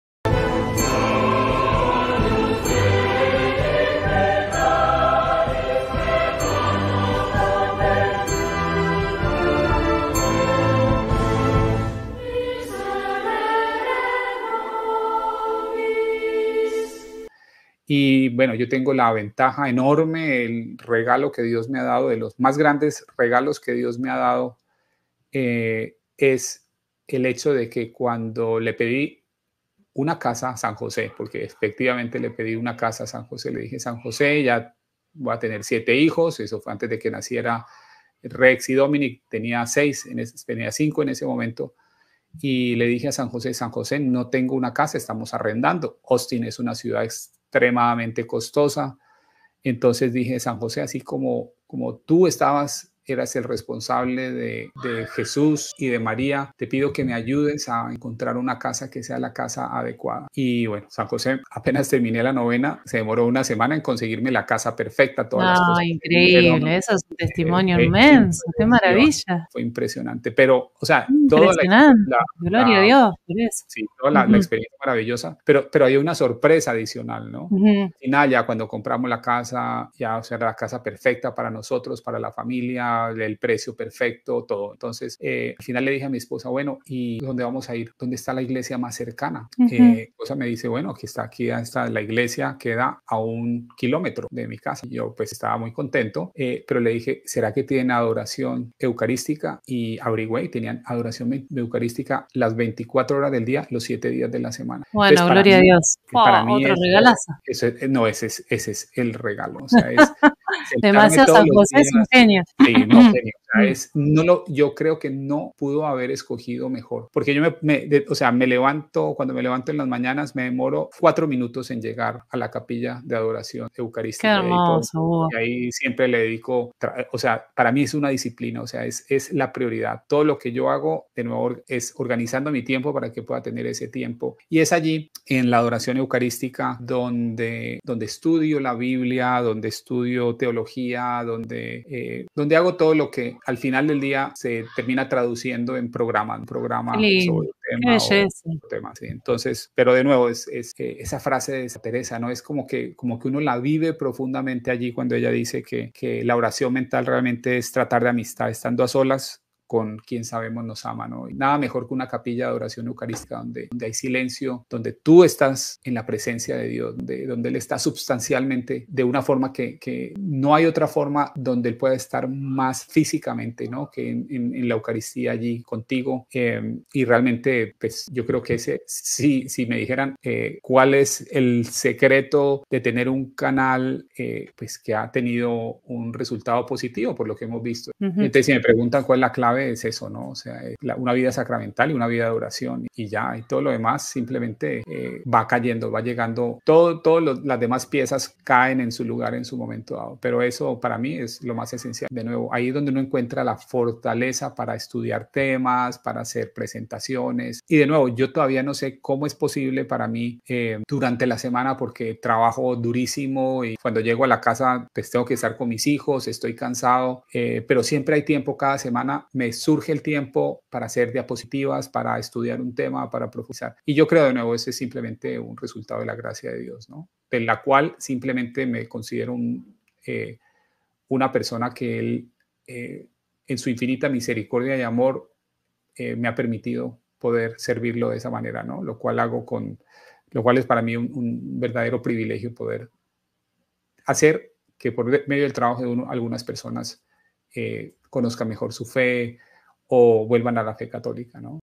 El Milagro que me hizo San José
En este programa cuento una historia muy personal de un milagro que recibí por la intercesión de San José. Si bien nosotros los católicos creemos que la fuente de todas las gracias, milagros y bendiciones que recibimos es Dios, él permite que apelemos a la intercesión de los Santos en nuestro favor, y esto es algo muy poderoso.